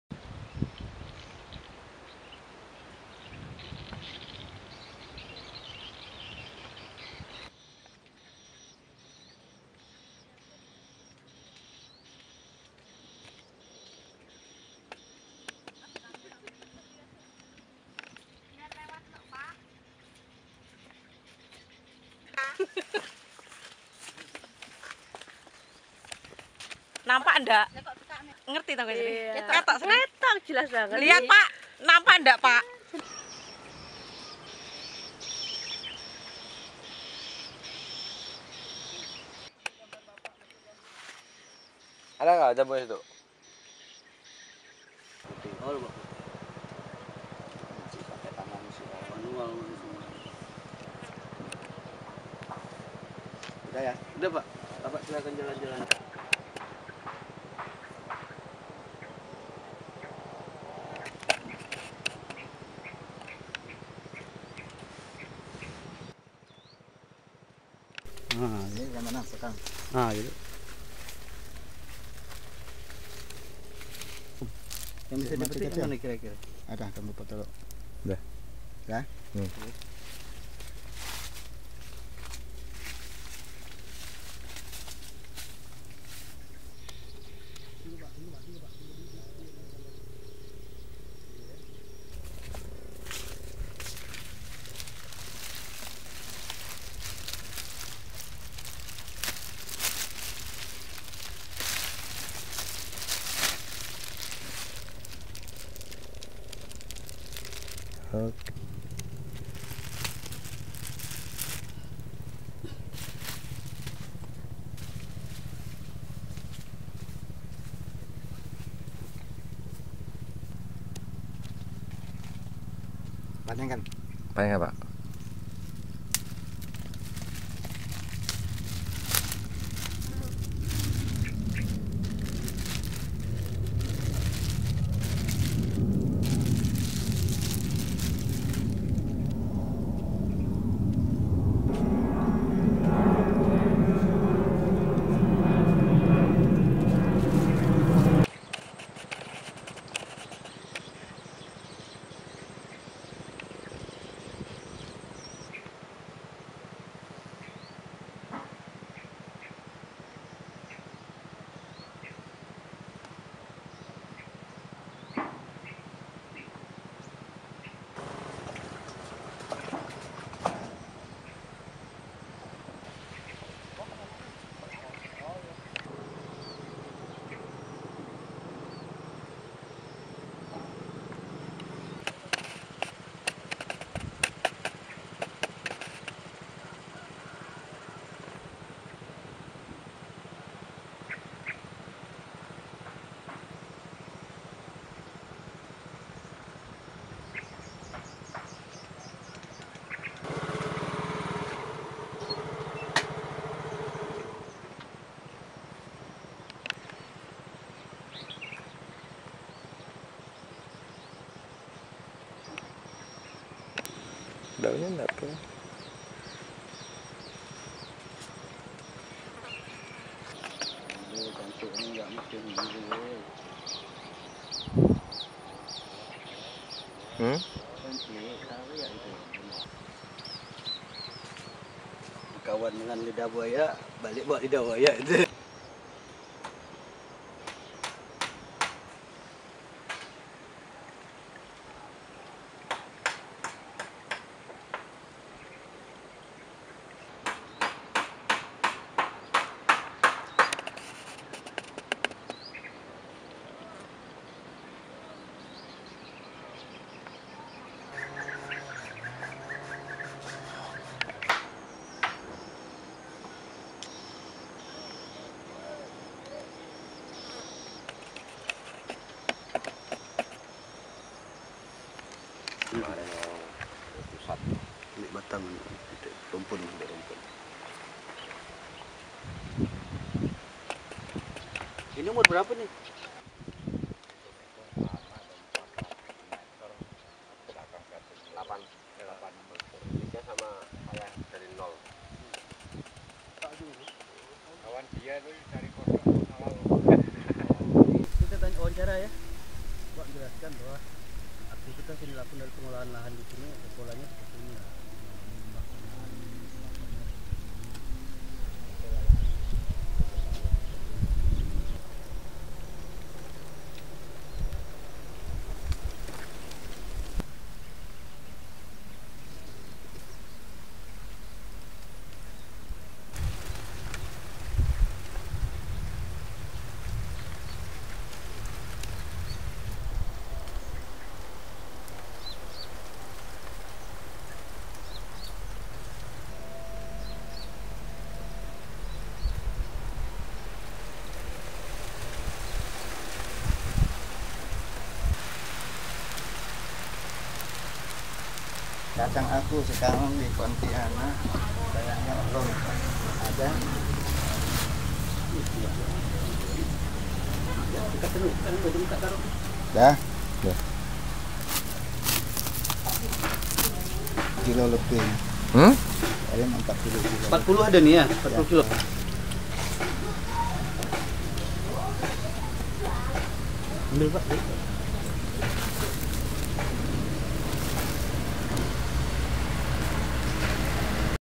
STOCKSHOT : KEBUN JAMBU KACANG TANAH DI LAHAN GAMBUT
Ternyata dilahan gambut bisa membudidayakan tanaman hortikultura dan tanaman lainnya